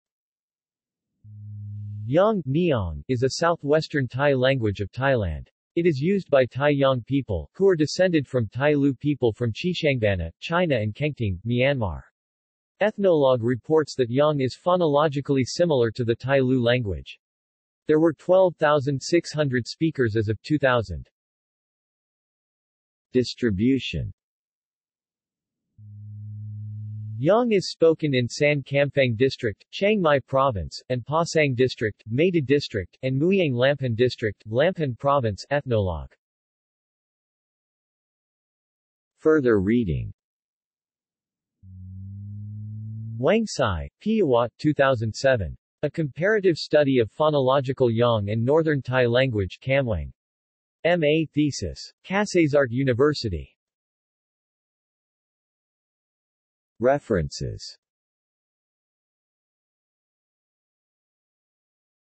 Yong language | Wikipedia audio article
This is an audio version of the Wikipedia Article:
Yong language

SUMMARY
Yong (Nyong) is a Southwestern Tai language of Thailand. It is used by Tai Yong people, who are descended from Tai Lue people from Xishuangbanna, China and Kengtung, Myanmar. Ethnologue reports that Yong is phonologically similar to the Tai Lue language. There were 12,600 speakers as of 2000.